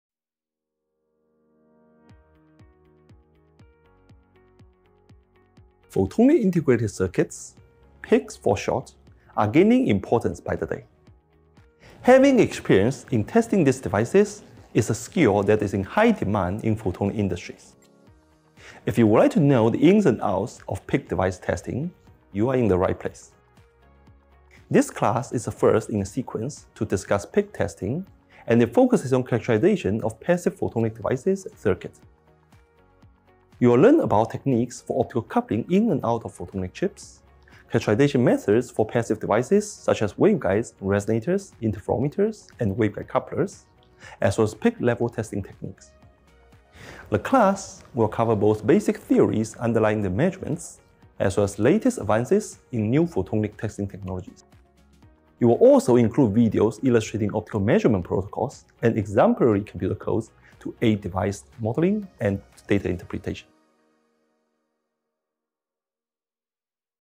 Integrated Circuits Testing: Passive Devices
Prof. Hu (MIT) reviews test characterization methods for passive integrated photonics components, including fiber-to-chip coupling schemes, waveguides, spirals, Mach Zehnder Interferometers, Y-splitters, ring resonators, and directional couplers. Waveguide characterization will focus on loss and dispersion measurements, and examine wavelength and polarization dependencies. Prof. Jaime Cardenas (U of Rochester) will offer his expertise in Design for Manufacturing (DfM) and testing at a wafer-scale. Register now!